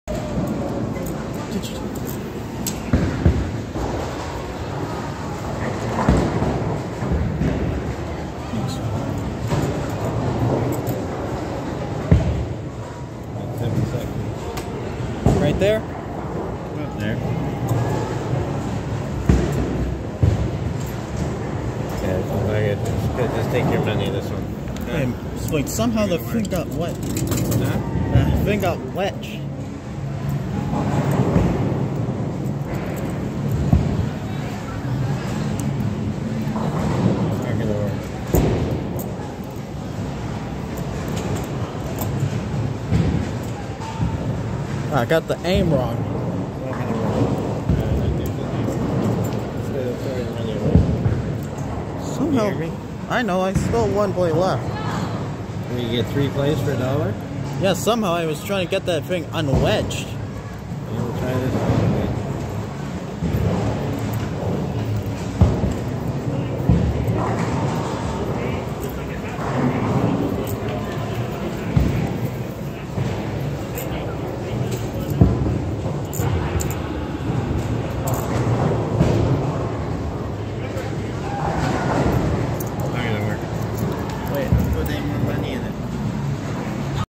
Claw machine adventures, episode 18. Father and son vs the claw machine
I was at a bowling alley playing the claw machine with my dad and a coin was stuck, but luckily enough I managed to push it in the machine and I got one extra play in claw machine to make it one play. Then I put two more coins after the extra play in this video.